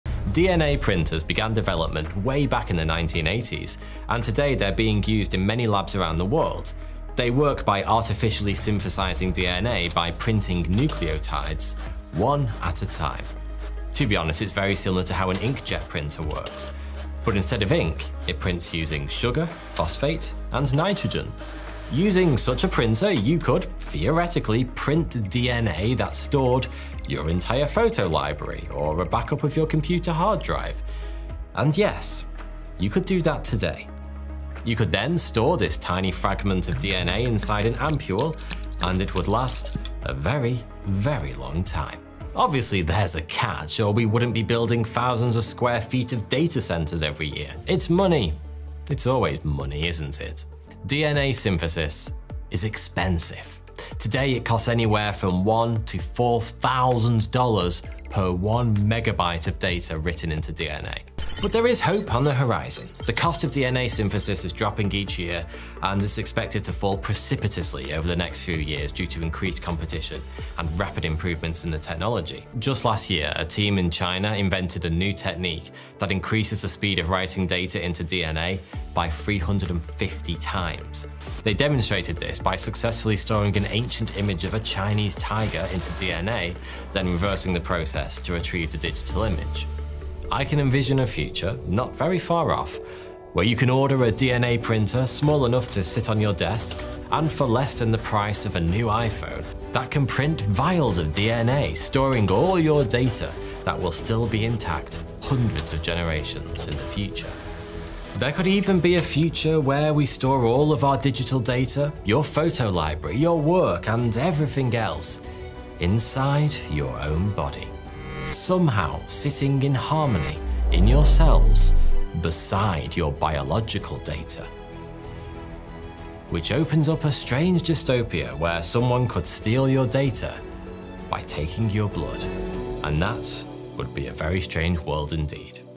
Could DNA Synthesis Be the Future of Data Storage, or Inspiration for a Future Back Mirror Episode?
DNA printers, which synthesise nucleotides, began development in the 1980s. Thanks to advances in technology and increases in affordability, there may be a future where all your data can be printed at a reasonable cost. But what could the consequences of that be?